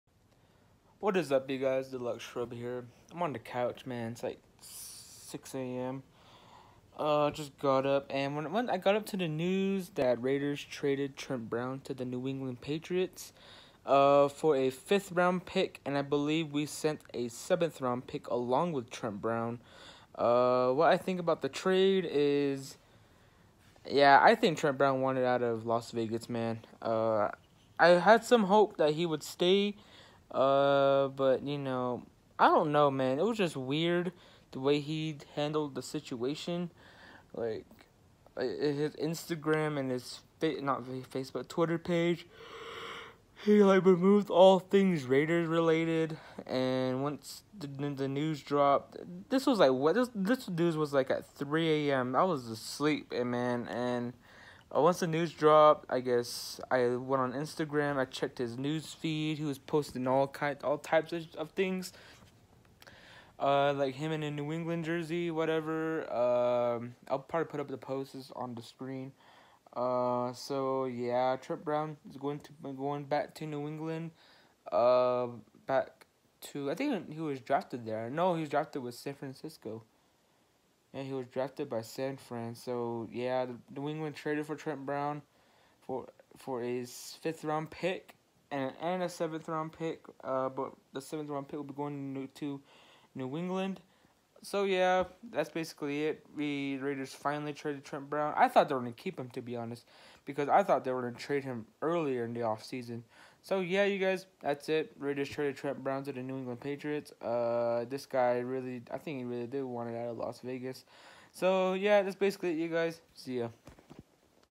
Raiders Traded T Trent Brown
Raiders trade Trent Brown and a 7th round pick to the Patriots for a 5th round pick.

This is DeluxeShruB signing off.